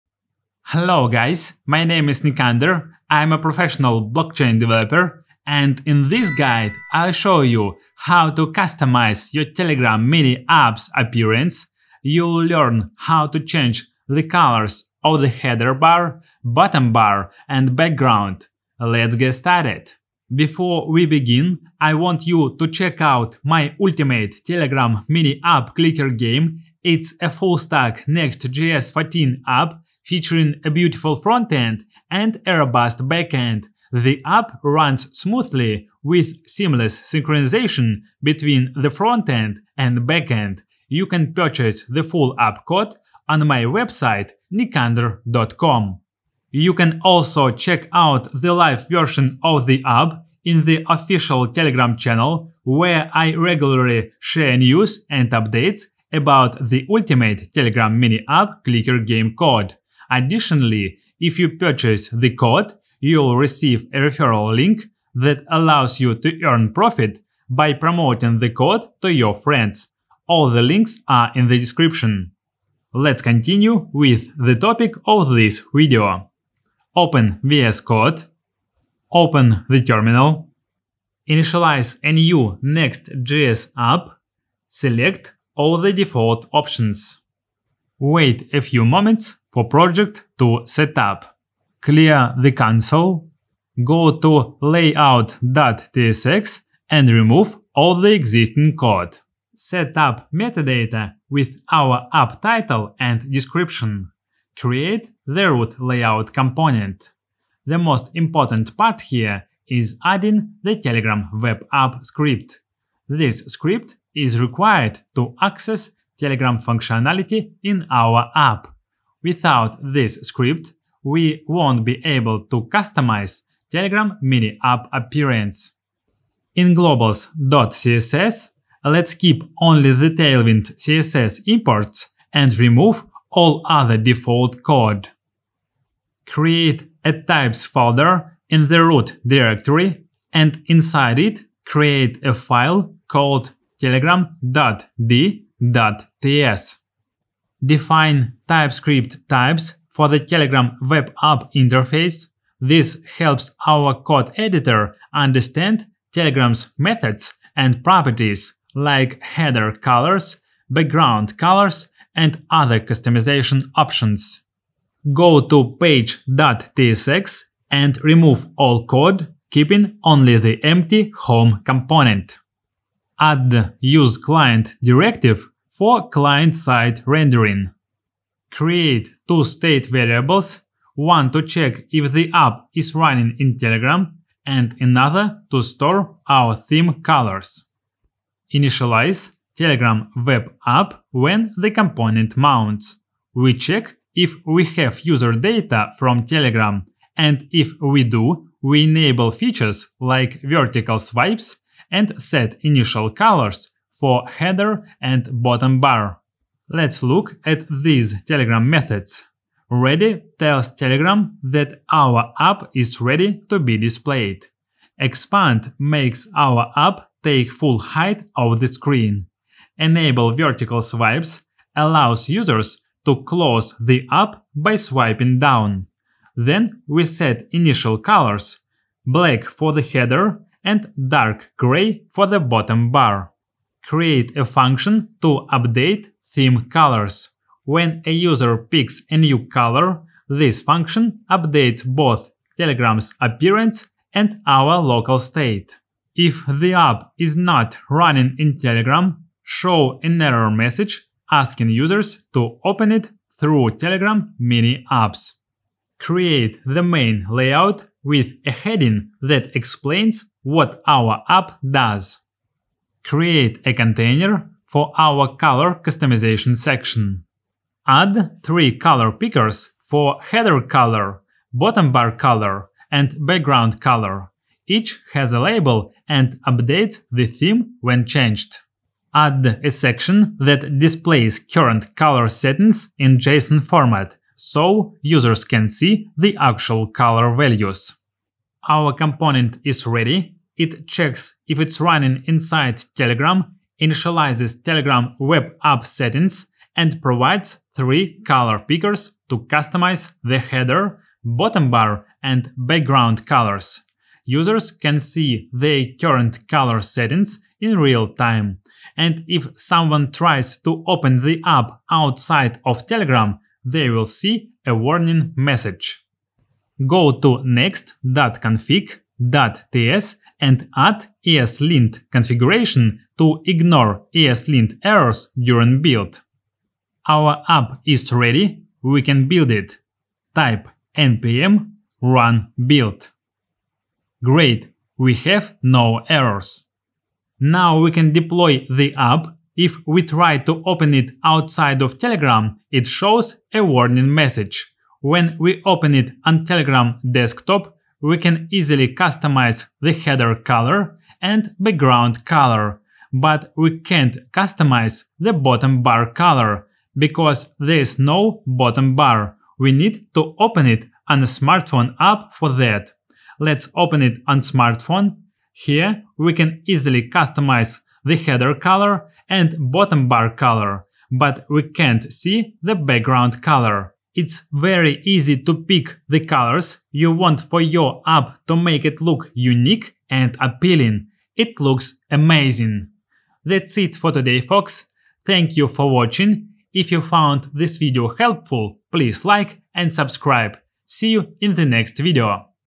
Make TON Telegram Mini App | 07 | Customize Telegram Mini App Theme
In this video, we'll create a Telegram Mini App using Next.js 14, TypeScript, and Tailwind CSS that demonstrates how to customize Telegram Mini App appearance. We'll implement color customization for header, bottom bar, and background, showing how to make your Telegram Mini Apps visually unique.

- Setting up a Next.js 14 project with TypeScript and Tailwind CSS
- Integrating with Telegram Web App API for theme customization
- Implementing color pickers for different UI elements
- Handling Telegram initialization and user validation
- Testing on both desktop and mobile Telegram clients

- Real-time color customization for Telegram Mini App UI
- Telegram environment detection
- Platform-specific UI behavior (desktop vs mobile)
- TypeScript type definitions for Telegram Web App

- Web Developers: Perfect for those building customized Telegram Mini Apps
- UI/UX Designers: Ideal for understanding Telegram Mini App theming capabilities
- TON Blockchain Developers: Great for those wanting to create visually appealing Telegram Mini Apps